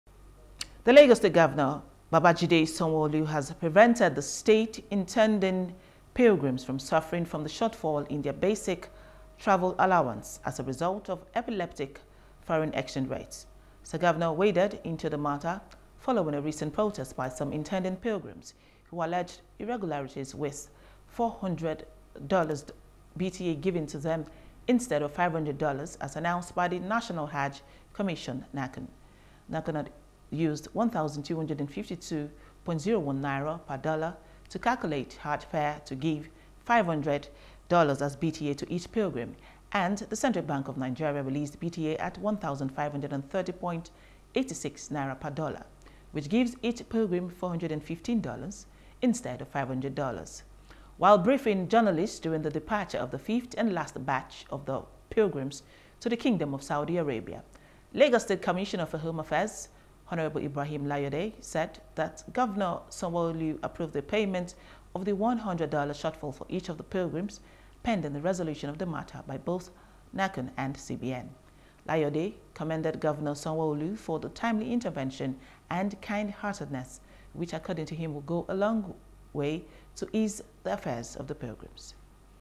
HAJJ 2024 GOVERNOR SANWO OLU RESOLVES BTA DIFFERENTIALS FOR PILGRIMS
Lagos State Governor, Mr Babajide Sanwo-Olu has prevented the state's intending pilgrims from suffering from the shortfall in their Basic Travel Allowance (BTA) due to epileptic foreign exchange rates.